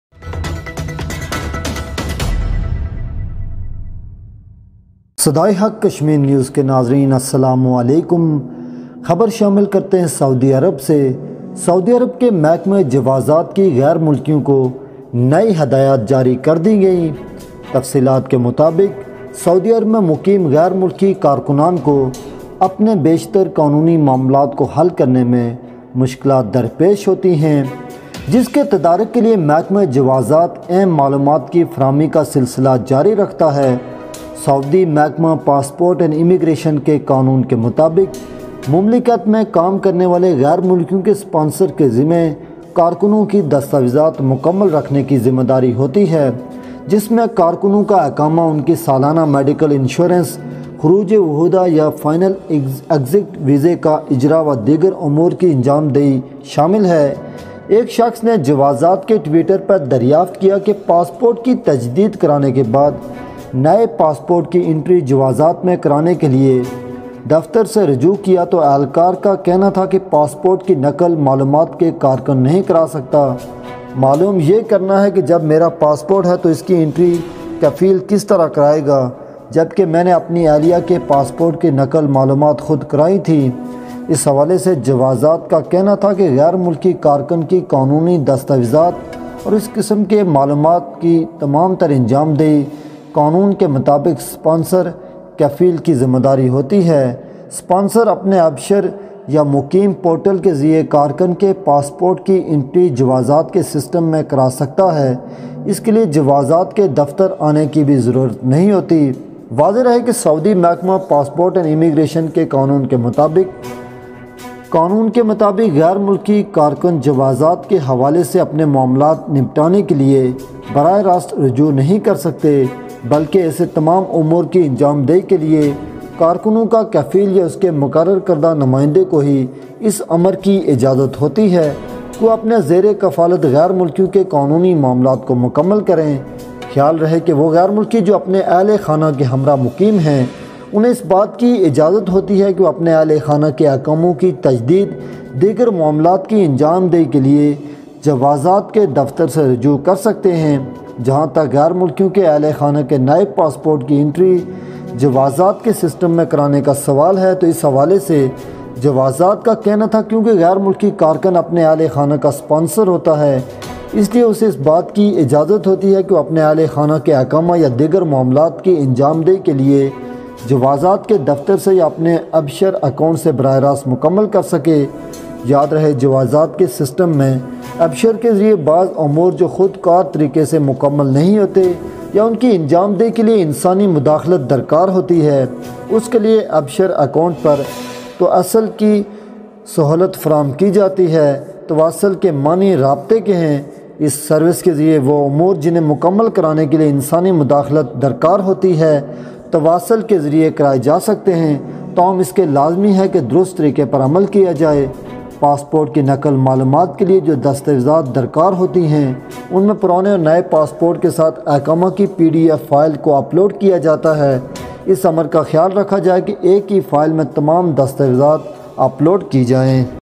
Saudi Arabia's Department of Permits issued new instructions to foreigners
Riyadh: Foreign workers living in Saudi Arabia face difficulties in solving most of their legal issues, for which the Department of Permits continues to provide important information.  Sponsors of foreign nationals are responsible for keeping the worker's documentation complete, including the worker's residency, annual medical insurance, issuance of exit visas or final exit visas, and other matters.

A person found out on the Twitter account of Juzaat that after renewing his passport, he approached the office to enter the new passport in Juzaat, the official said that the passport copy worker cannot do it. It is to be known that when my passport  If so, how will the sponsor do his entry while I had made a copy of my wife's passport myself? In this regard, Juzaat said that the sponsor according to the law to carry out the legal documents of the foreign worker and similar matters.

 The sponsor can enter the worker's passport in the permit system through its Abshar or resident portal, for this there is no need to come to the permit office. It should be noted that according to the law of the Saudi Department of Passport and Immigration, foreign worker permits are issued according to the law.  They cannot approach directly to deal with their affairs, but for the execution of all such matters, the sponsor of the workers or his appointed representative is allowed to complete the legal affairs of the foreigners under their custody.

 It should be noted that those foreigners who are residing with their families are allowed to approach the Juzaat Office to renew the residences of their families or to perform other matters. As far as the families of foreigners are concerned,  Regarding the question of entering the new passport of the family in the system of permits, in this regard, the permits said that because the foreign worker is the sponsor of his family, he is allowed to do the iqama or other matters of his family.  can be completed directly from the Juzaat office or from your Abshar account.

 It should be noted that in the authorization system, some issues which are not completed automatically or require human intervention through Abshar, the facility of 'Tawaswal' is provided on the Abshar account. Tawaswal means communication.  .  Through this service, those issues that require human intervention to complete can be done through communication, but it must be done correctly.

 The documents required for duplicate passport information are uploaded PDF file of Iqama along with old and new passport keys, care should be taken to upload all the documents in one file.